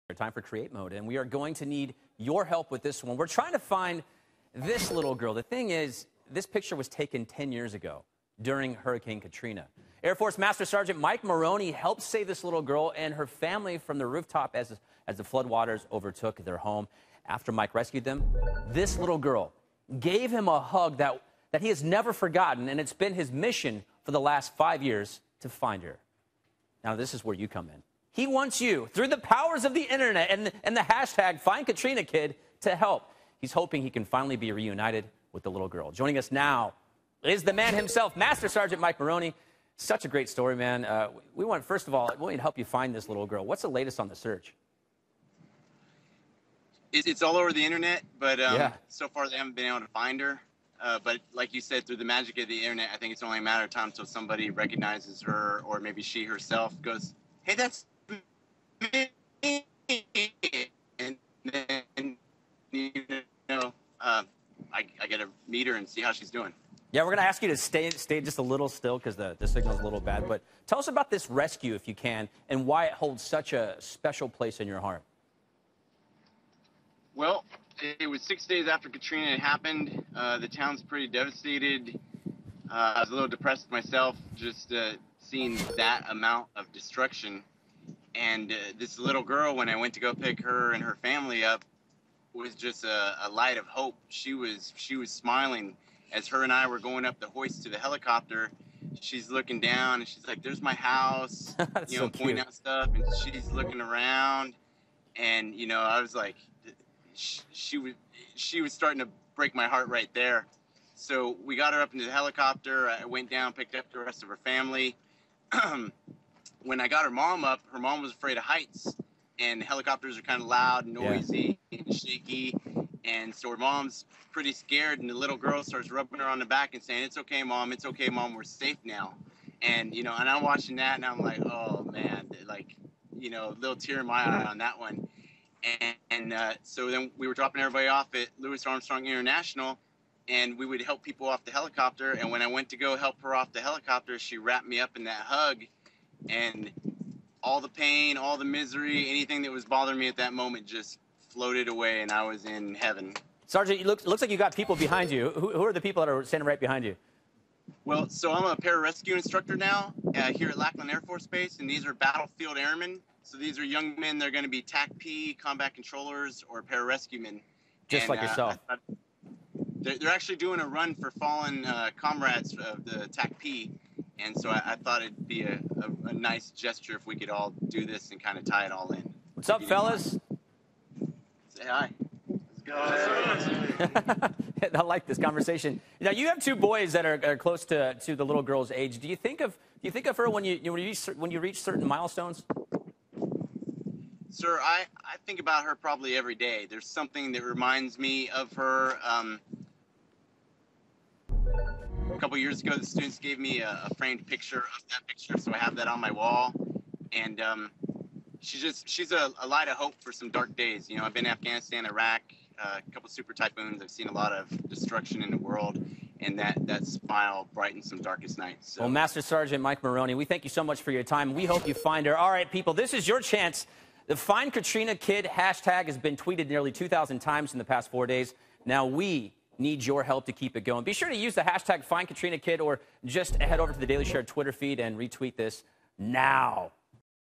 Veteran explains his Internet quest to FindKatrinaKid
Master Sgt. Mike Maroney has been involved in hundreds of rescue missions during his 18 years with the Air Force, but it's one special rescue that has never left his mind. Maroney is on a mission to find the little girl that gave him a ray of hope during the dark days following Hurricane Katrina.